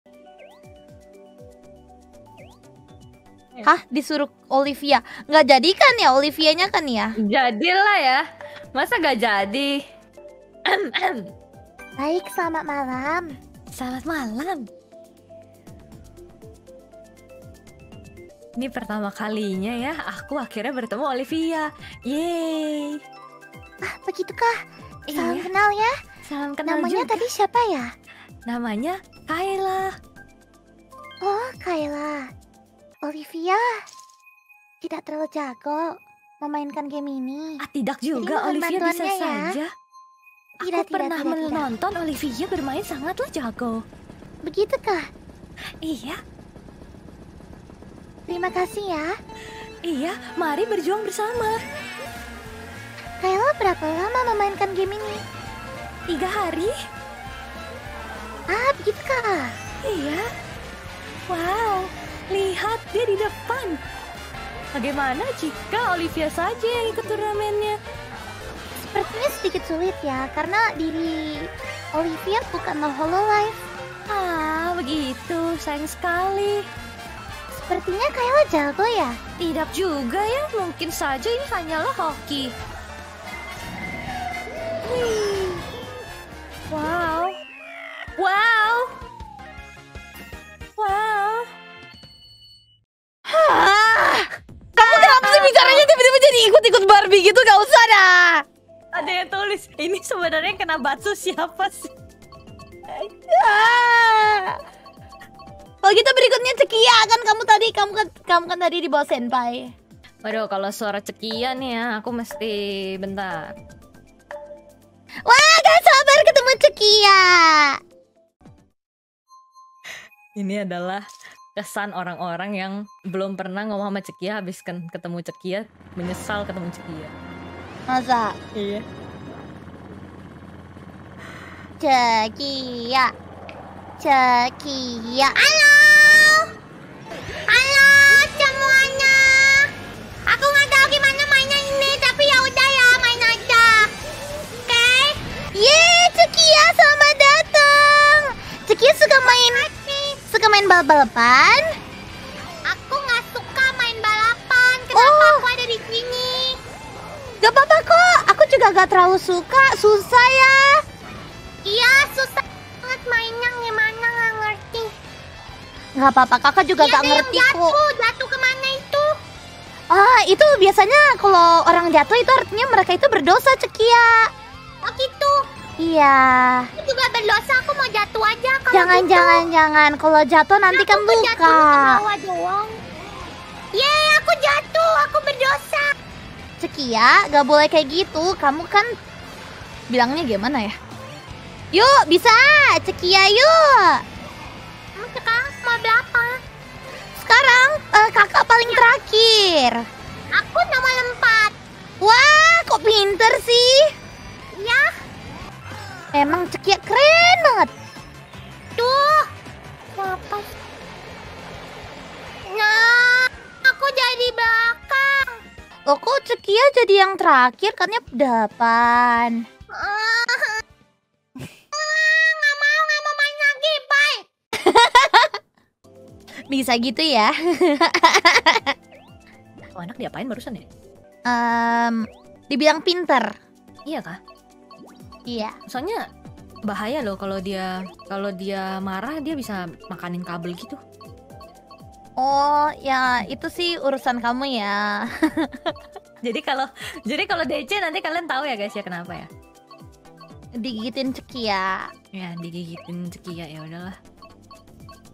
[2 POV] Olivia ketemu Kaela diisengin, Ckia ketemu Ollie kayak ngasuh anak『HoloClip ID』
wkwkwk